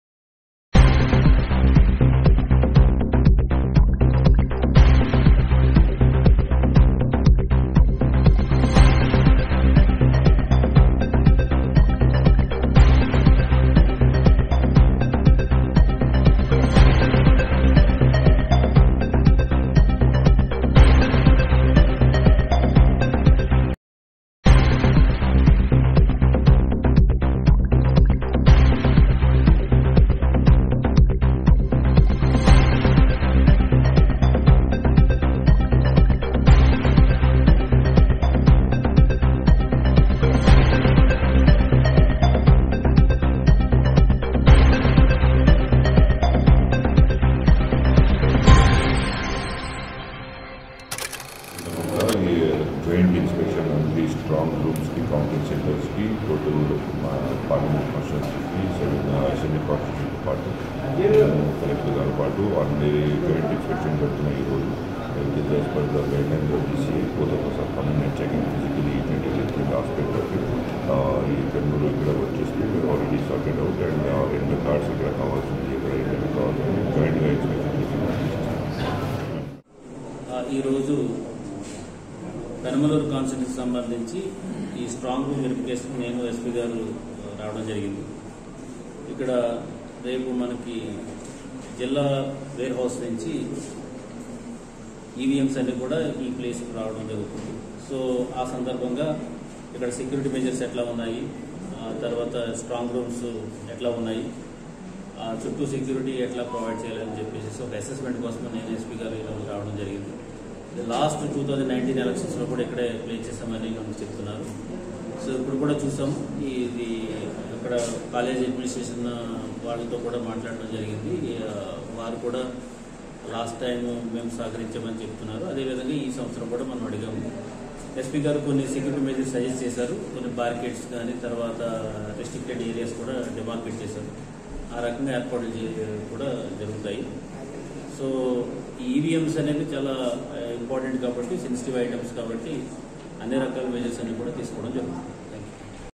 కృష్ణాజిల్లా దనేకుల ఇంజనీరింగ్కళాశాలలో జిల్లాకలెక్టర్ రాజాబాబు SP అద్నాన్ నయూమ్ అస్మి ఆకస్మిక తనిఖీ
కృష్ణా జిల్లా పెనమలూరు నియోజకవర్గం గంగూరిలోని దనేకుల ఇంజనీరింగ్ కళాశాలలో ఎన్నికల నేపథ్యంలో భద్రత ఏర్పాట్లు అలాగే స్ట్రాంగ్ రూమ్ సంబంధించిన ఏర్పాట్లను జిల్లా కలెక్టర్ రాజాబాబు ఎస్పీ అద్నాన్ నయూమ్ అస్మి పరిశీలించారు. ఈ సందర్భంగా కలెక్టర్ మాట్లాడుతూ జిల్లా వేర్హౌస్ నుంచి ఈ కళాశాలకు ఈవీఎం మెషిన్లు రావడం జరుగుతుంది అని అందుకోసం స్ట్రాంగ్ రూమ్ భద్రత ఏర్పాట్లు పరిశీలించడం జరిగింది అని తెలిపారు